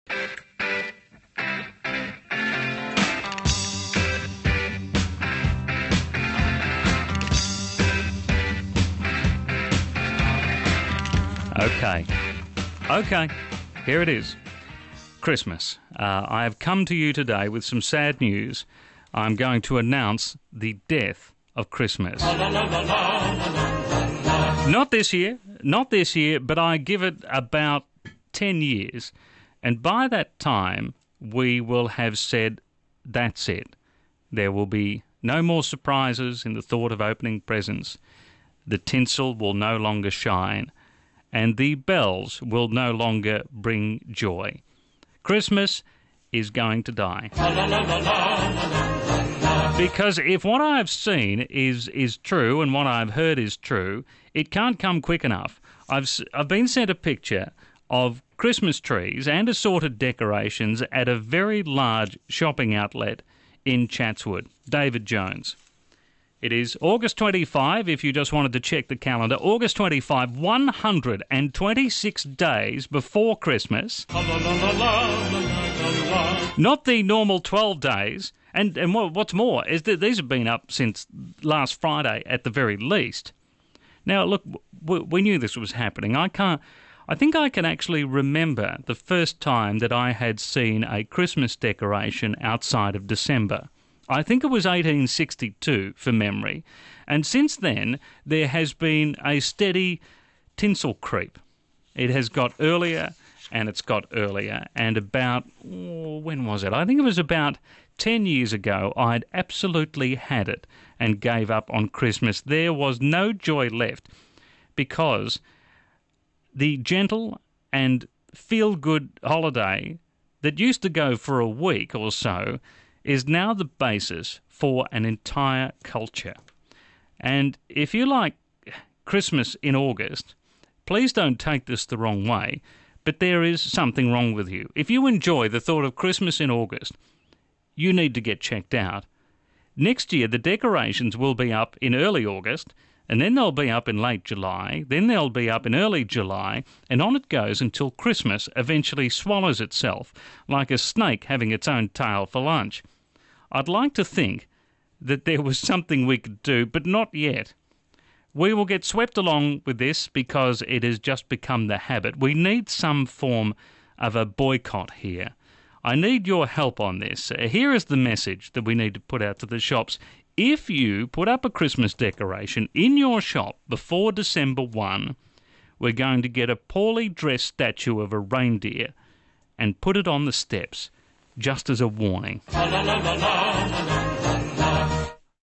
Justin Smith Video Editorial - Tinsel Creep: why Christmas is dead!
Christmas as we know it is dead. Retail has killed the joy, and has slowly destroyed what was a magic time in December. Justin Smith on tinsel creep. Are you seeing decorations already in your shops?